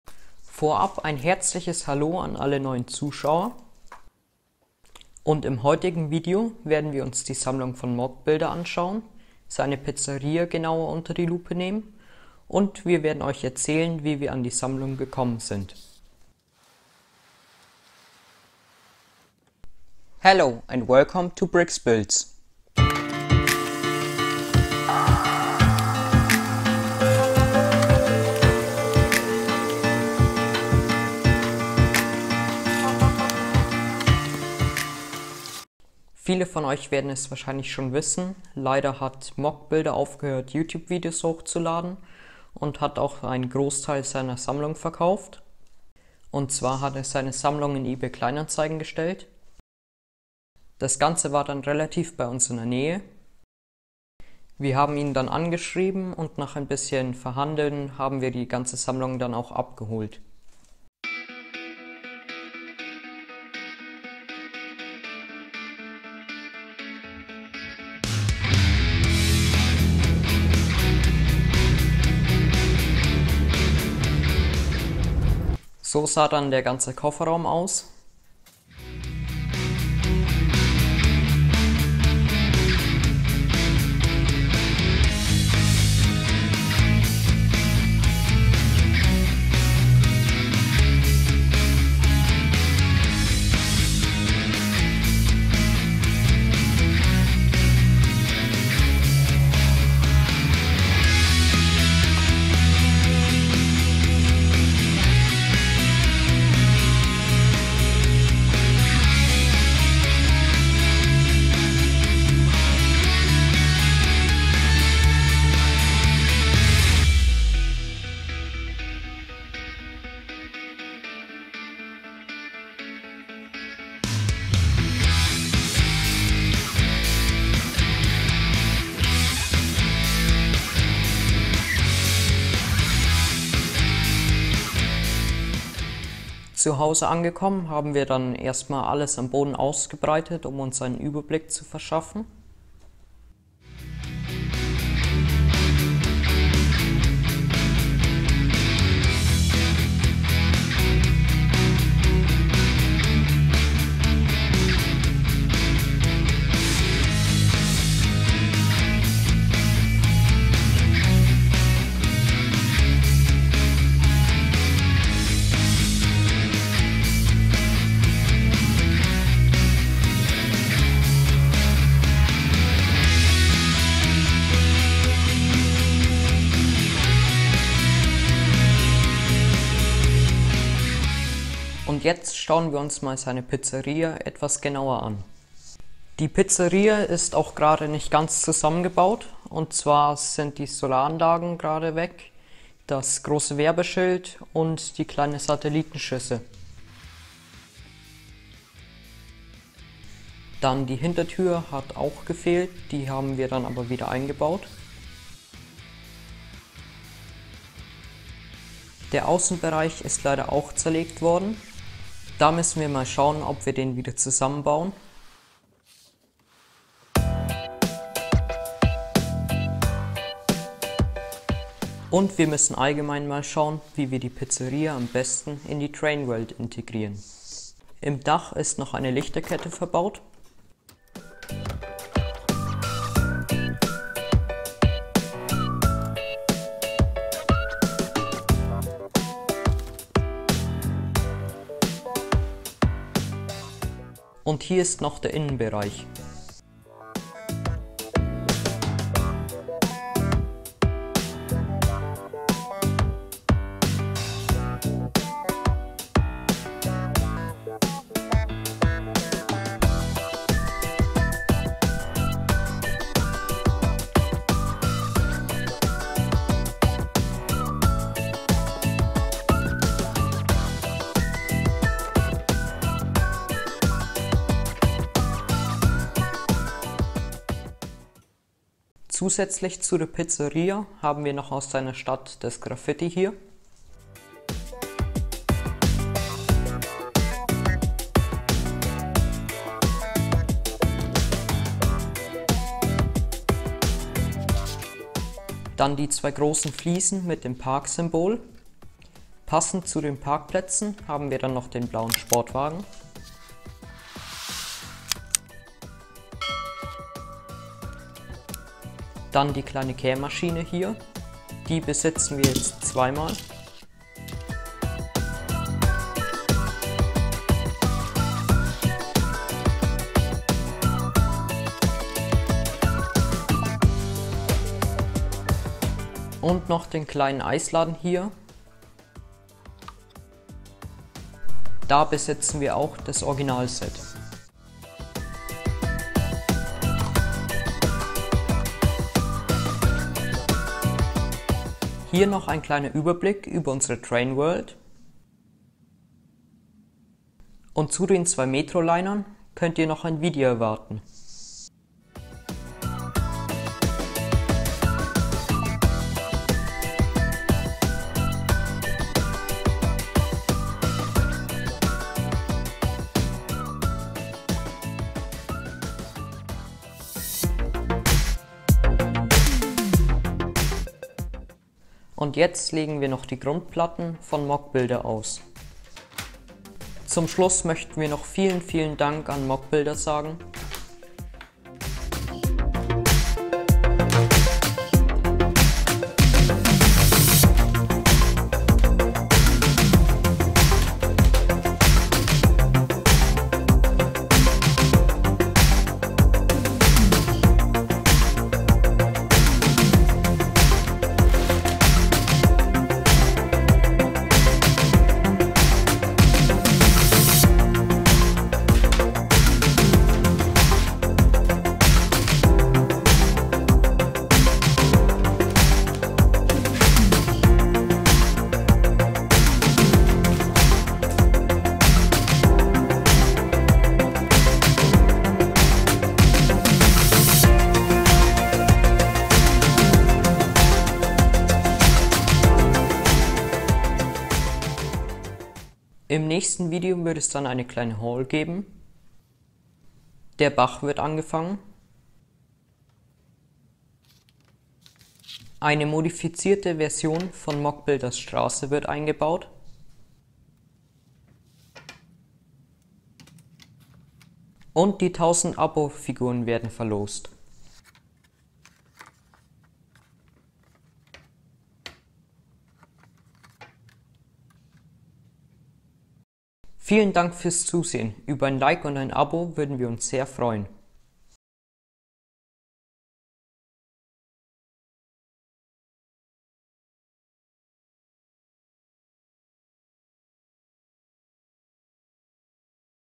Lego Pizzeria von Moc Builder - Review seiner Teile - Erweiterung unserer Legostadt
Im heutigen Video zeigen wir, wie der Kontakt zu moc builder entstanden ist, wie wir dann die Lego-Teile bei im abgeholt und danach bei uns gesichtet haben. Außerdem war ein tolles modular building moc (eine Lego Pizzeria) mit dabei, welche wir auch gerne zeigen. Darüber hinaus haben wir nun begonnen, die Teile in unsere Train World bzw. Lego Stadt einzubauen. Viel Spaß!

Wir auf discord:
0:00​ - Intro
0:05 - Inhalt
0:35 - Teile moc builder
3:02 - Die Pizzeria
4:41 - weitere Teile
7:15 - Vorschau
8:00 - Outro